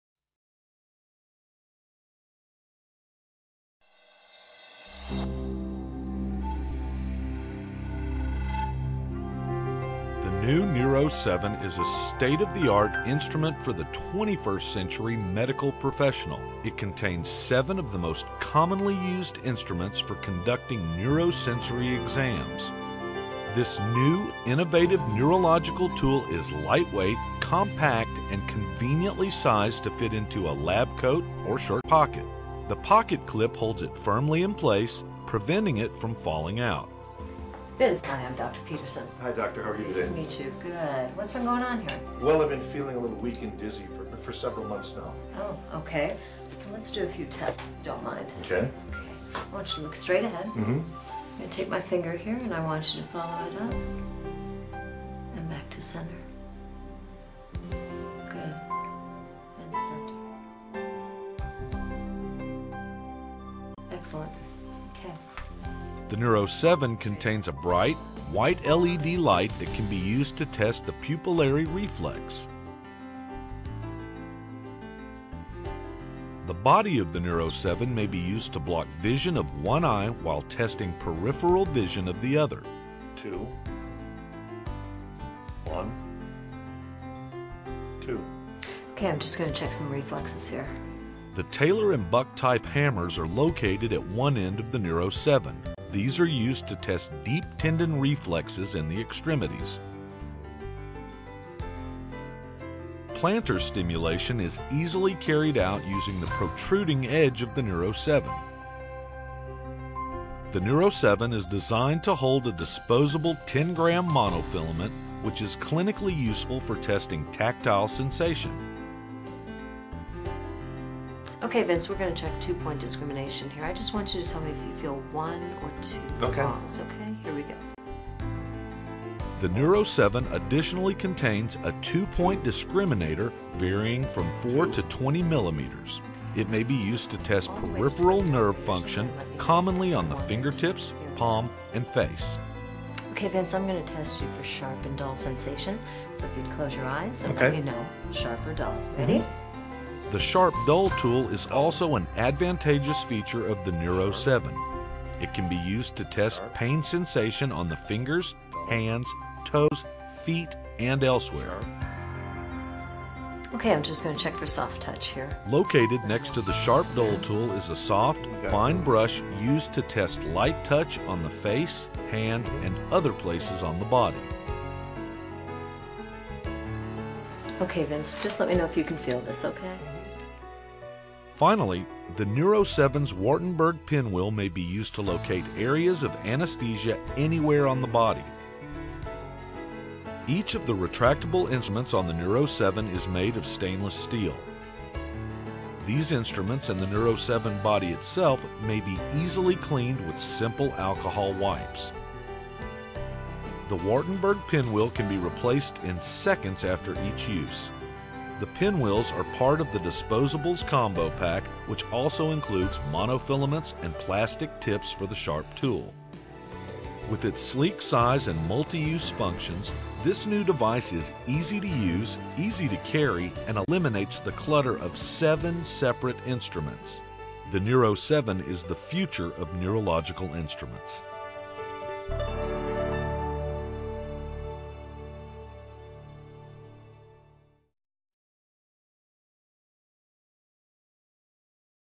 Neurological instrument for consistent, quality exams | Neuro7
All-in-one instrument featuring a reflex hammer, pupil gauge & penlight, wartenberg pinwheel, two-point discriminator, & other neurological sensory testing tools.